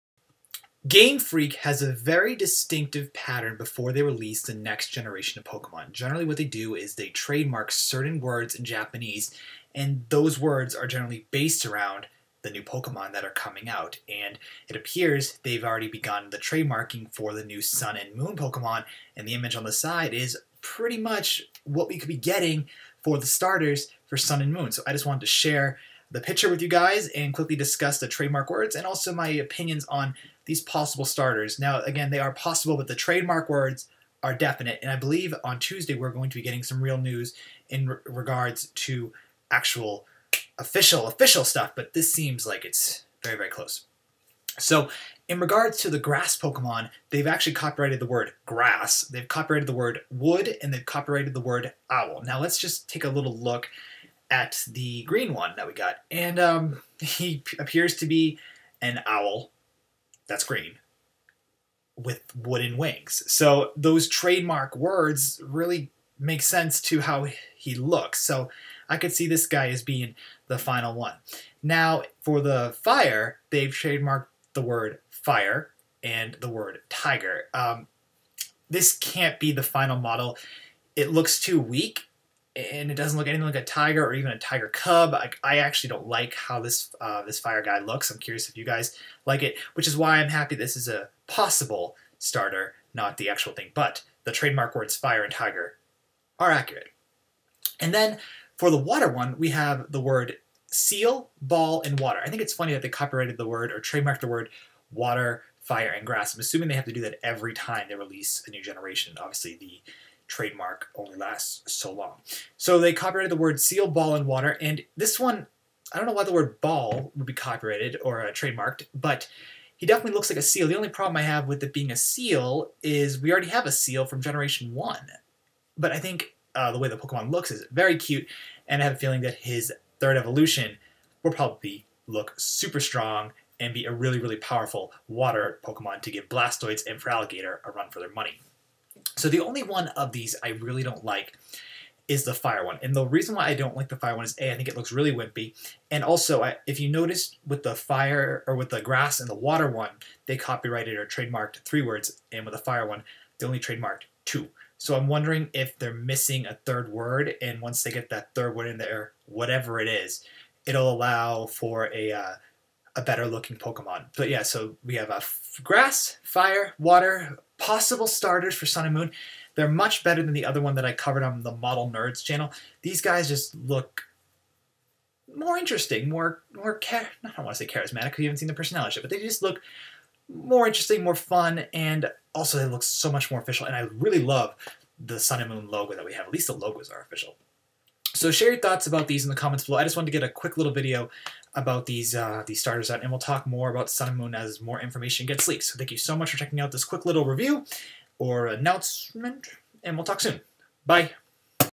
Pokemon Sun and Moon: New Starter Pokemon Artwork Leak?|RandJReviews
Could these be the offical Sun and Moon Starters? Here is Rob's review of the art work!

If you wanna talk more to Joe and Rob make sure to connect off Youtube